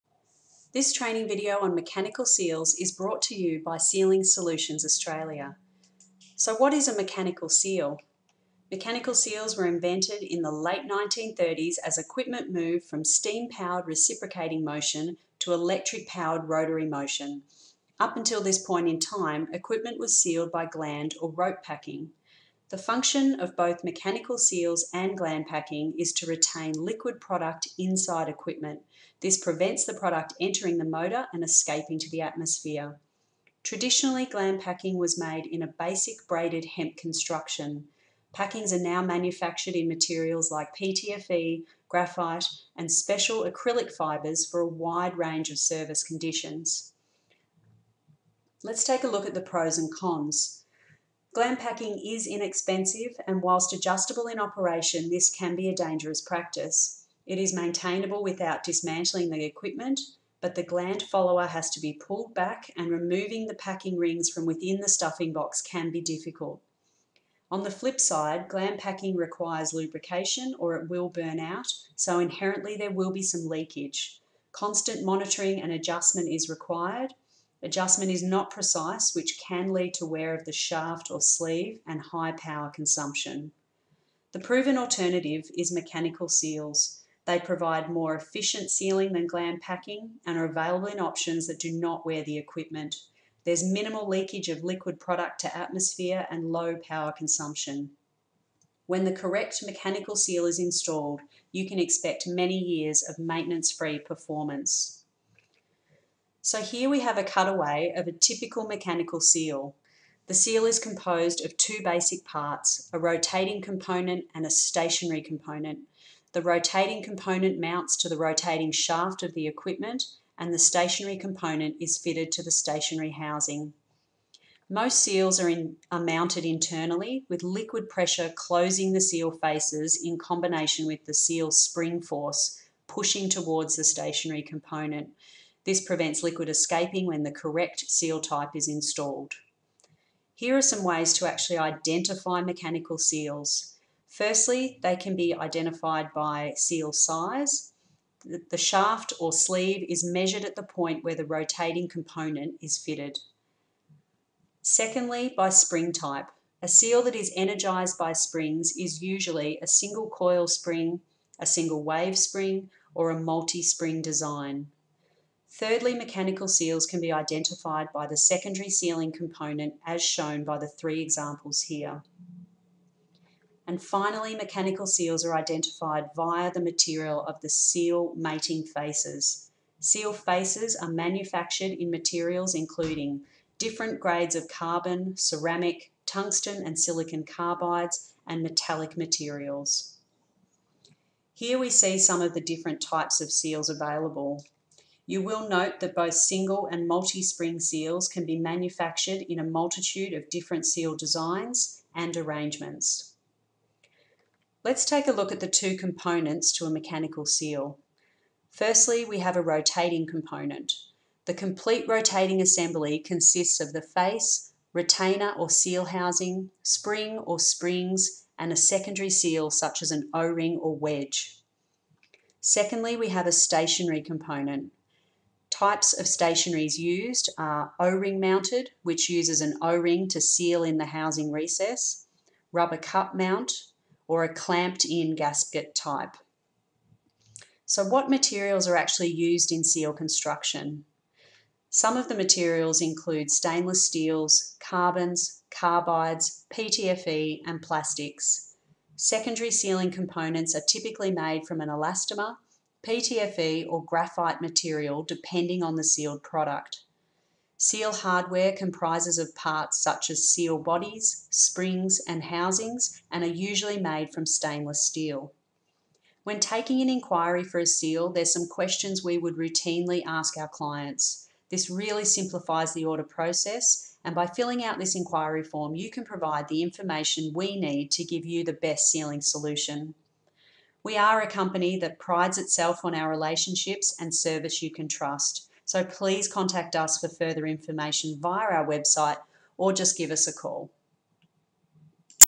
What Is A Mechanical Seal?
The function of Mechanical Seals is to retain liquid product inside equipment which in turn prevents product entering the motor and escaping to the atmosphere.